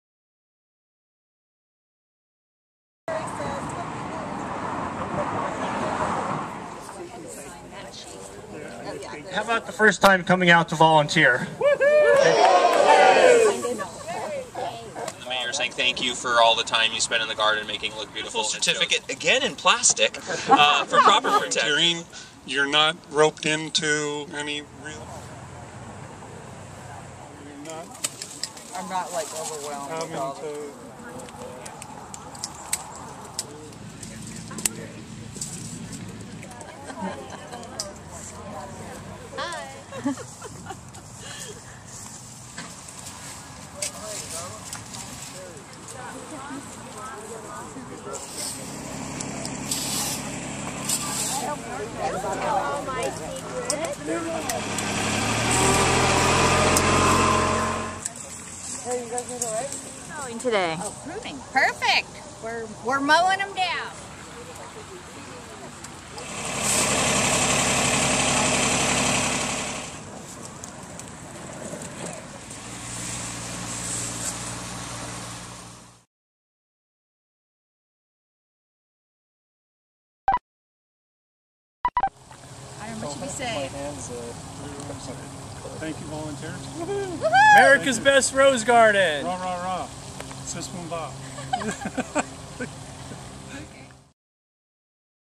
Shear Power - Annual Pruning of Rose Garden
175 volunteers of Friends of the San Jose Rose Garden prune 4,000 rosebushes by noon, using methods of "pruning for maximum display."  For more information on Friends of the San Jose Rose Garden, a nonprofit volunteer organization for America's Best Rose Garden